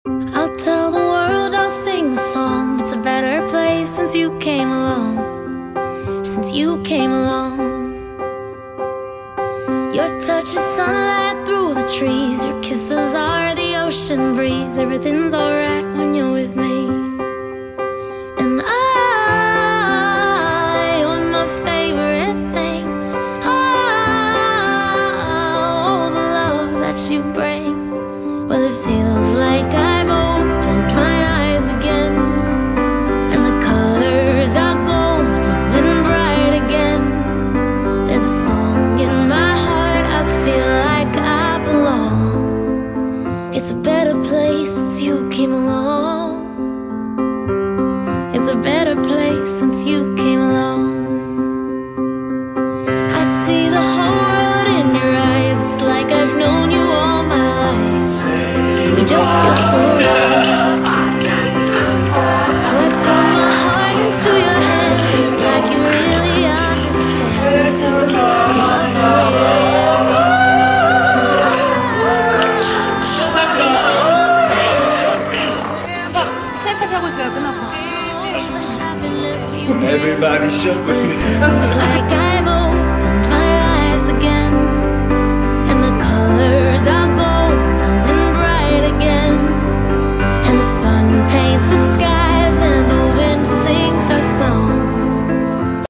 Happy Birthday Actor Youn Junghoon! 연정훈 배우의 생일을 축하합니다~
Junghoon's pictures all over the years, since childhood until now ♡